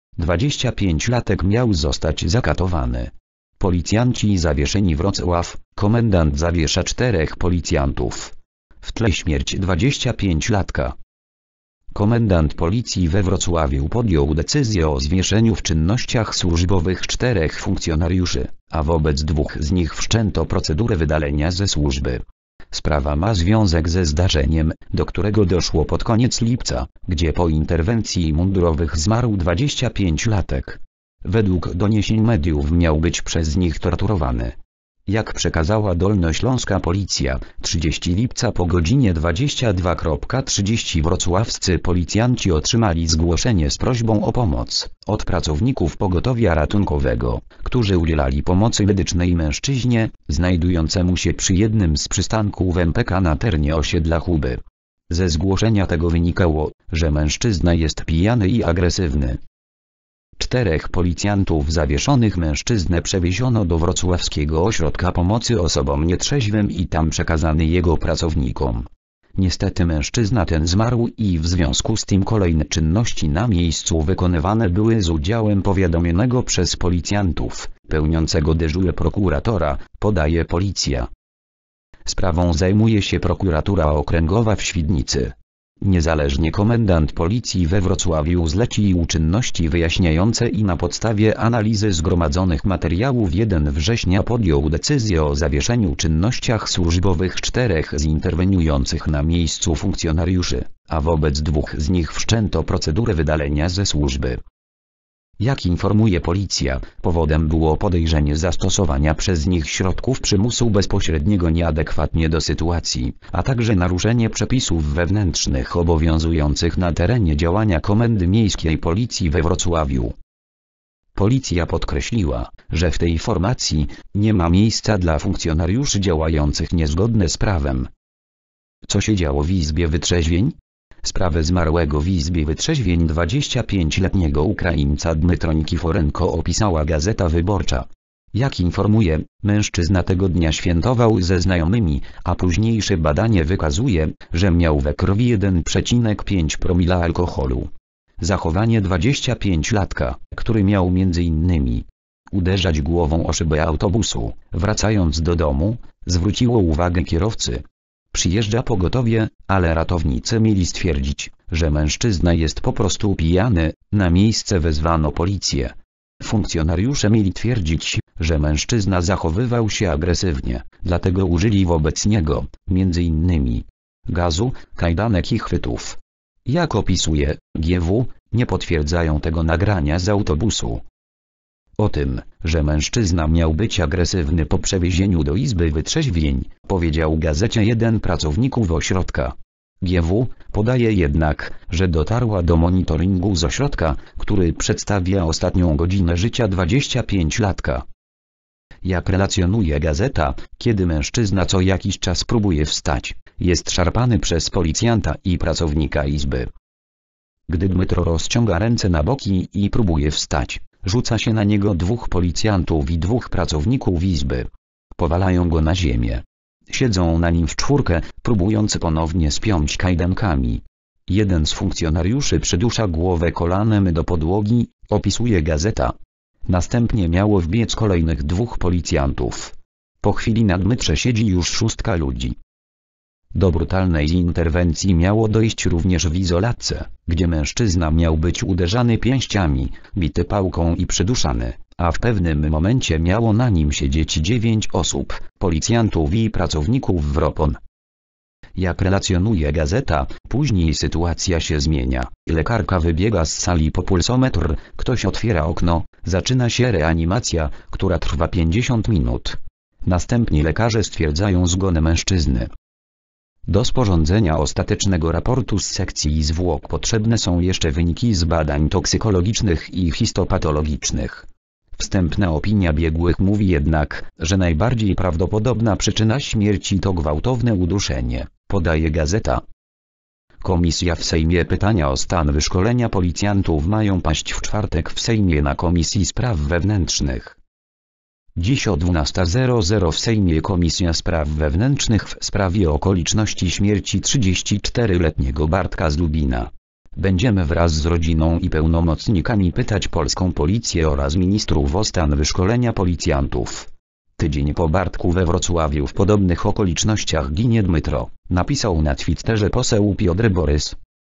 25-latek miał zostać zakatowany. Policjanci zawieszeni.
Wrocław: Komendant zawiesza czterech policjantów. W tle śmierć 25-latka.

   (fot. Shutterstock/DarSzach)